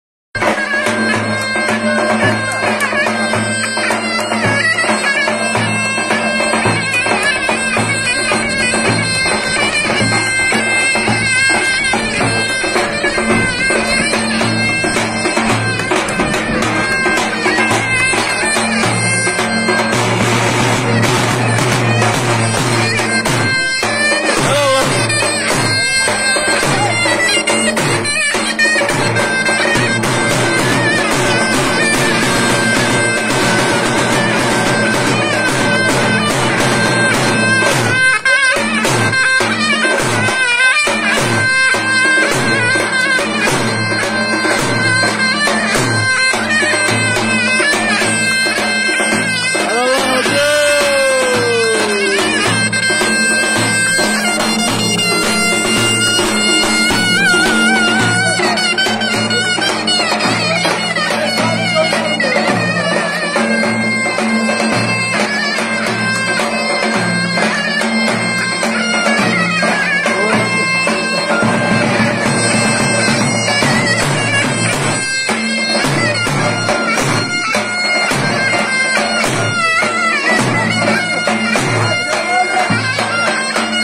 Sümer mahallesi düğünden bir halay ( Taçınlılara helal olsun)
Sümer mahallesi düğünden bir halay ( Taçınlılar olmasa halimiz harap... Bünyan davula zurnaya halaya hasret vallaha) Teşekkürler Cesur Aydın